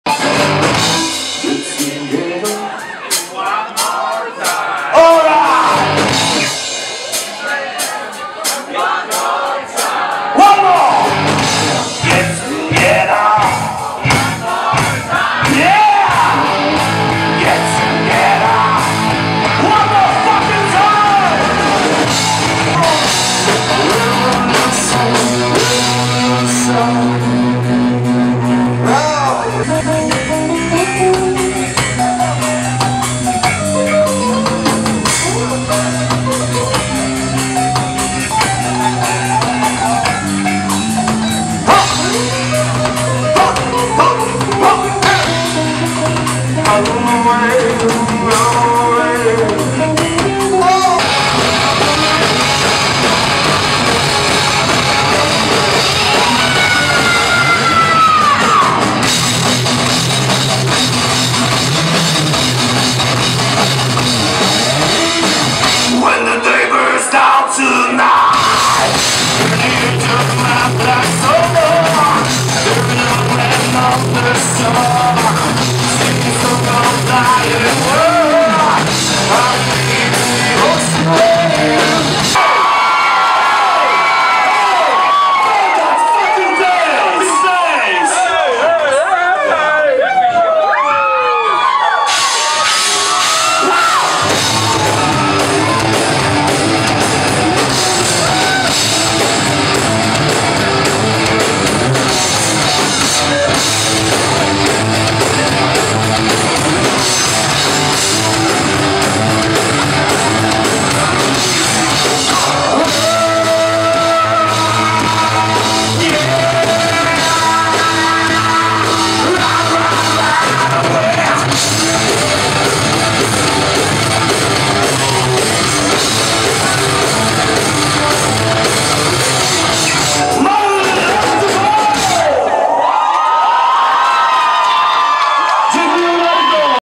Paweł Małaszyński z Cochise w Iron Horse
W piątek wieczorem, w pubie Iron Horse wystąpił założony przez aktora Pawła Małaszyńskiego zespół Cochise.